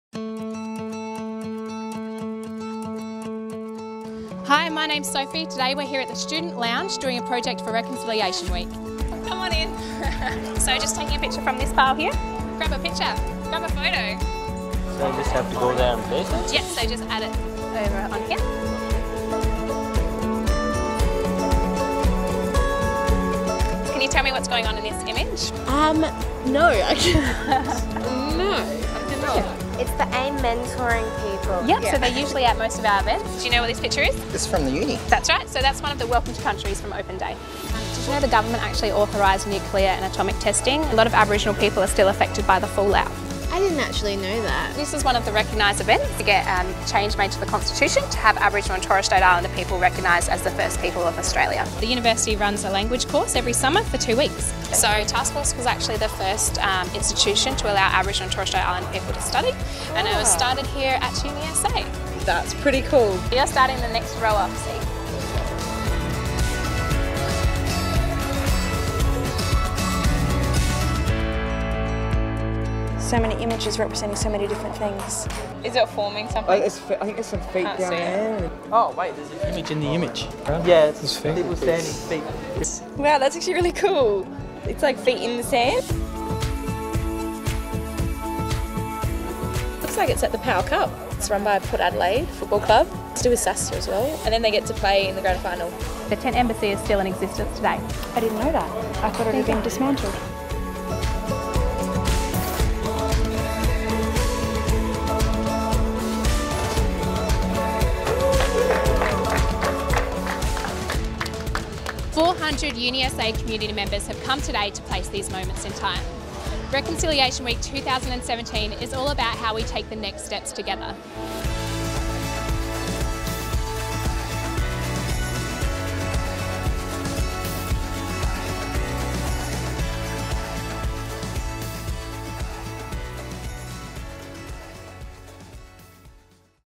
Reconciliation Week 2017 – Let’s Take the Next Steps
The theme for National Reconciliation Week 2017 is ‘Let’s Take the Next Steps’. Watch as we take a look back at key moments in history and work together as a UniSA community to take the next steps with reconciliation.